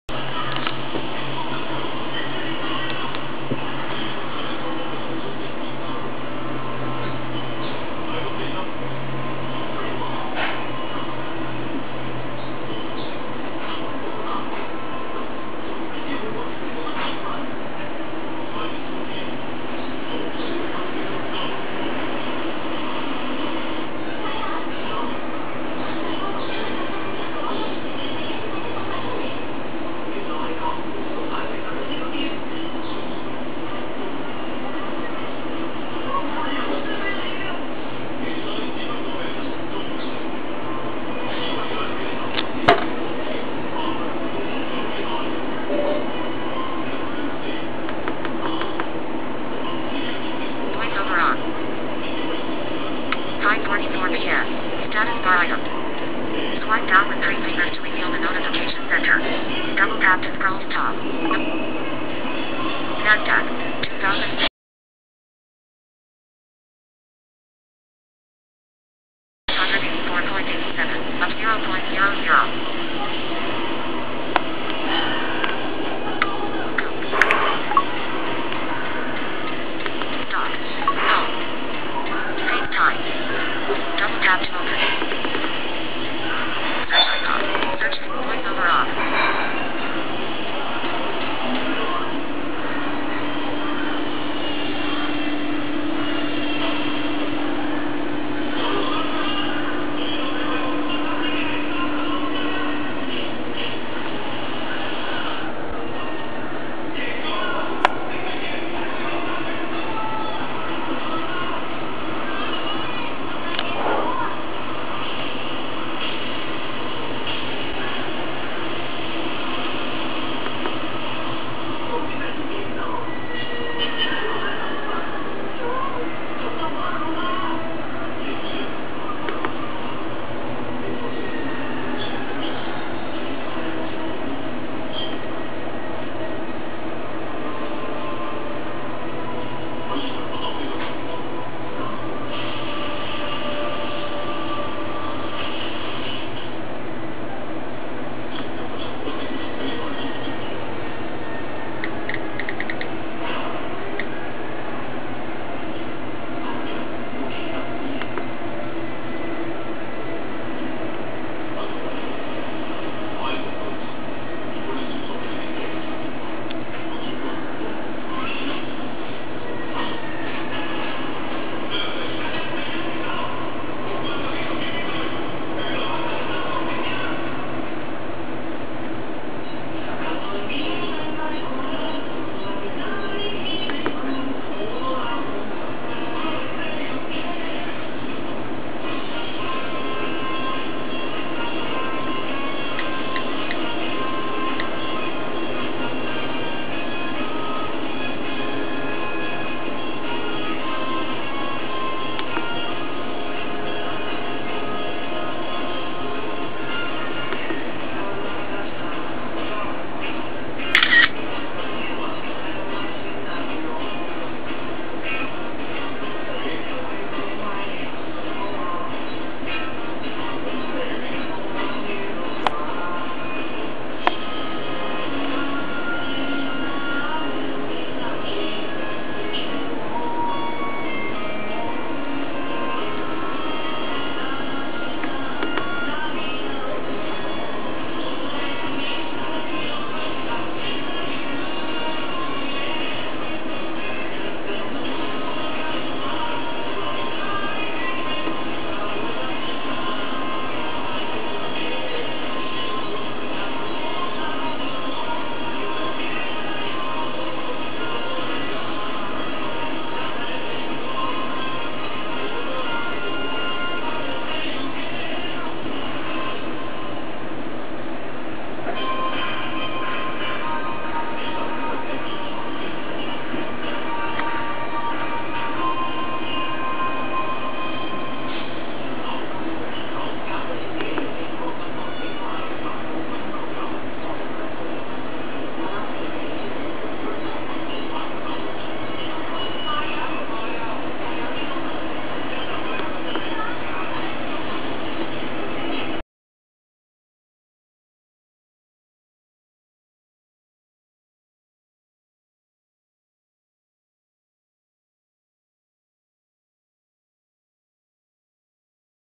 [iOs 5 beta] Activate iOs 5 without UDID/Developer account + Review [iPod Touch]
Install iOs 5 beta
1. Google the iOS 5 beta ipsw and iTunes 10.5 beta
2. Backup your device
3. Using the "Restore" function of itunes 10.5 to restore iOs beta to your device
4. Follow the video instriction to  activate iOs 5 without UDID/Developer account